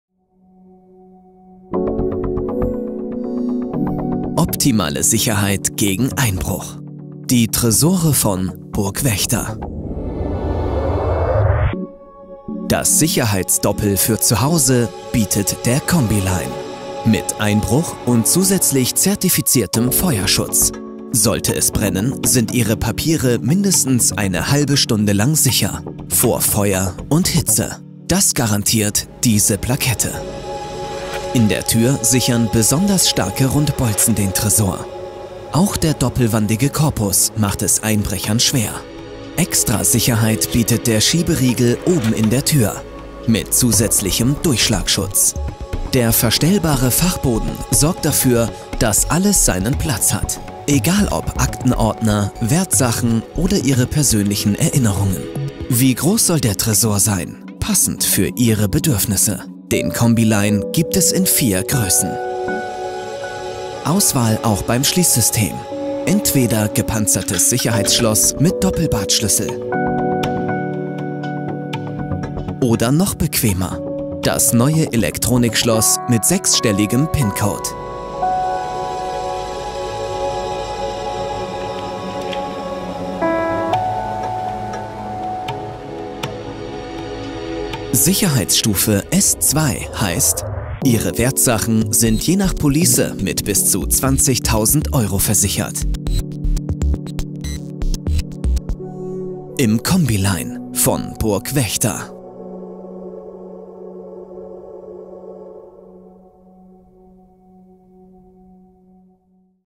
Combi-Line von BURG-WÄCHTER: Der Tresor mit Doppel-Schutz
Zertifizierter Einbruchschutz trifft zertifizierten Feuerschutz: Die neuen Combi-Line Tresore von BURG-WÄCHTER bietet Privatnutzern wie Firmen doppelte Sicherheit. Erhältlich in vier Größen und wahlweise mit modernem elektronischem Code-Schloss oder dem klassischen Schlüssel.